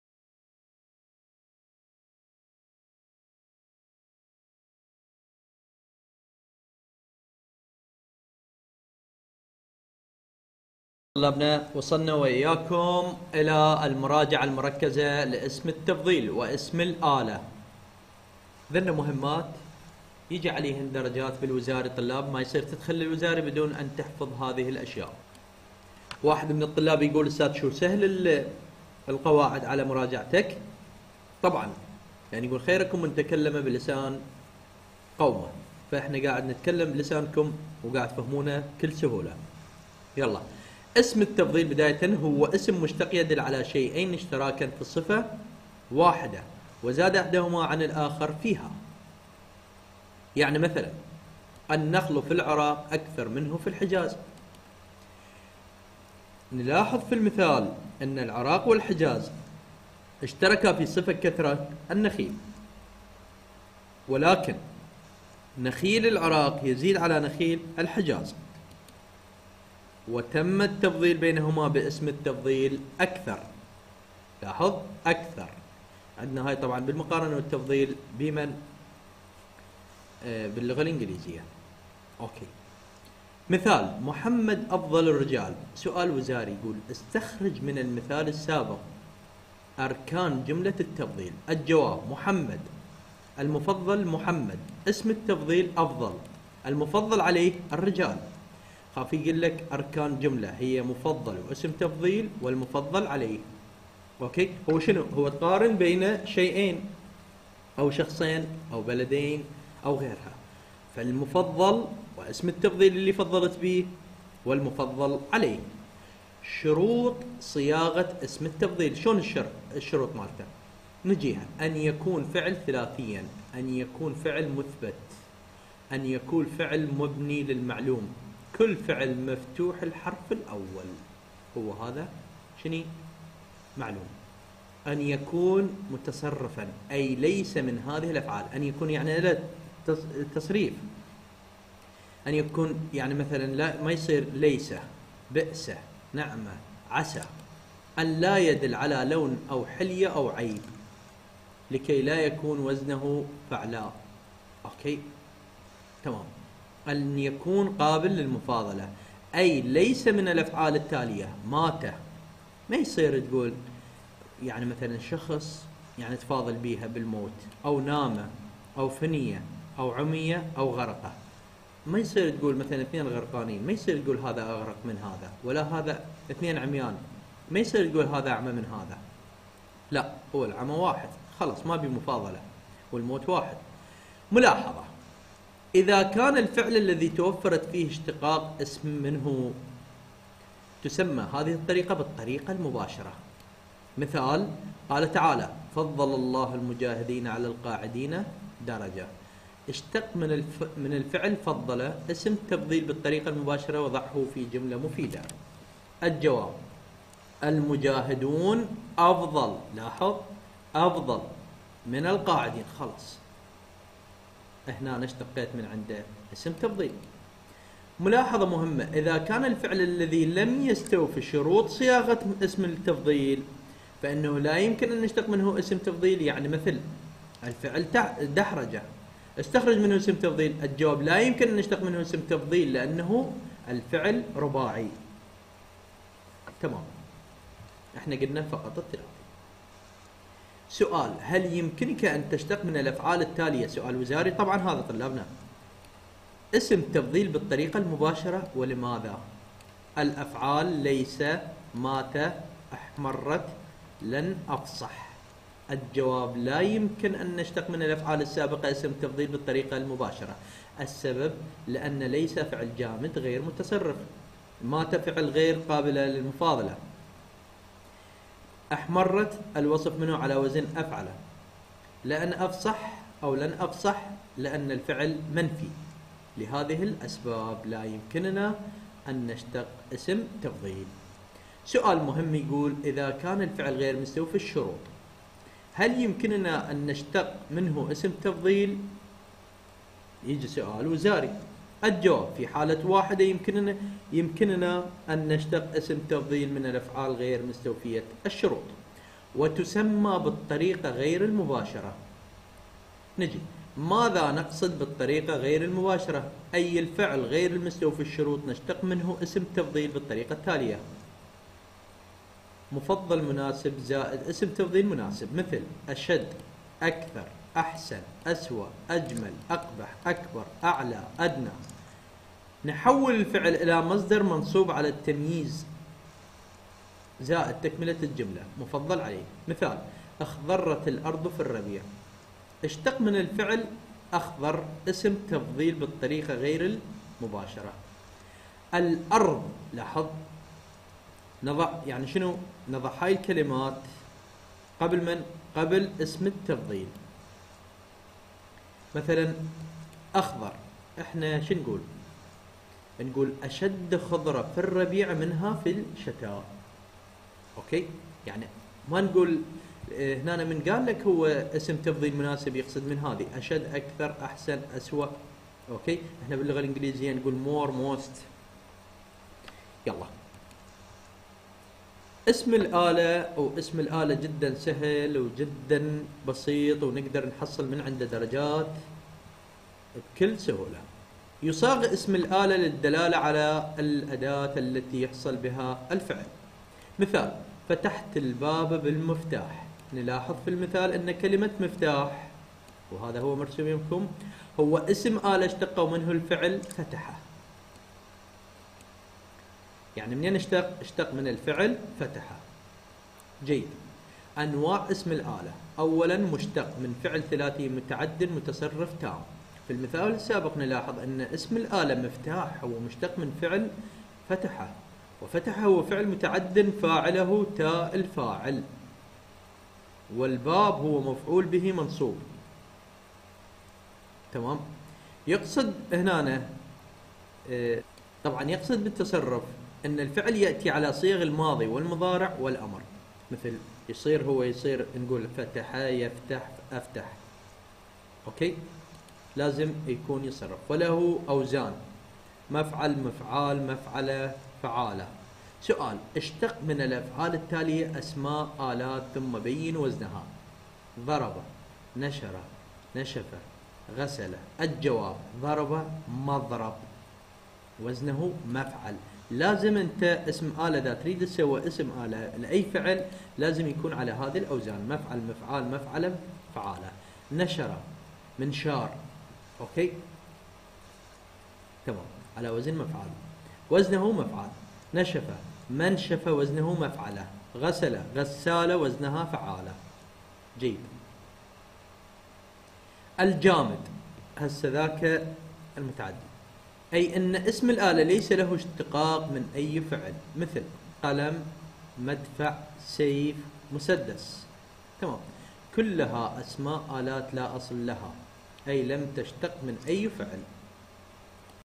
اللغة العربية، الثالث متوسط، المحاضره الرابعة، موضوع مهم جداً، اسم التفضيل،موضوع، اسم الالة،وزاريات
قنوات لطلاب الخارجي/الصباحي/جميع المراحل 💯✌